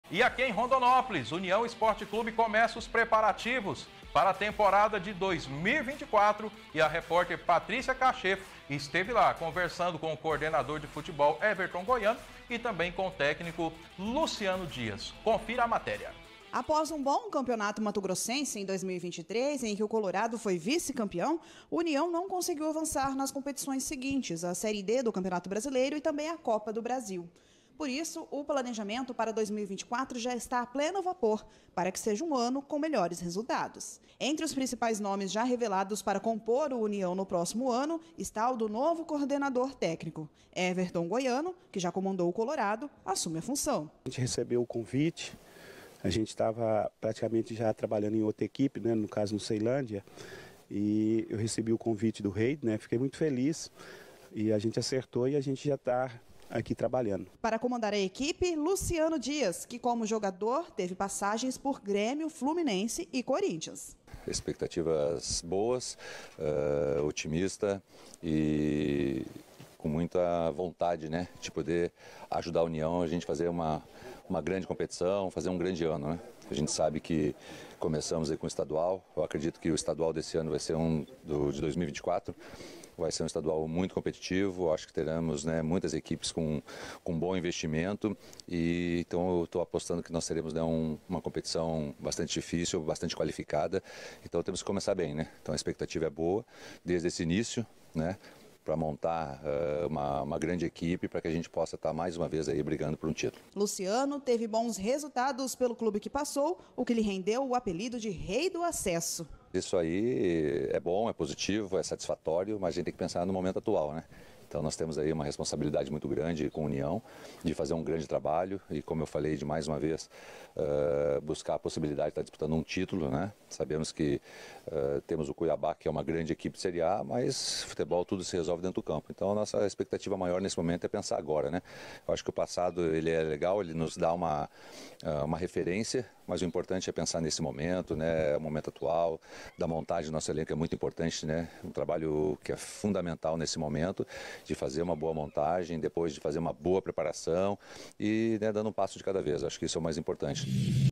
Everton Goiano e o técnico Luciano Dias fala da expectativa da temporada 2024 👊🏽🇦🇹
@SIGA UNIAOFANATICO NO INSTAGRAM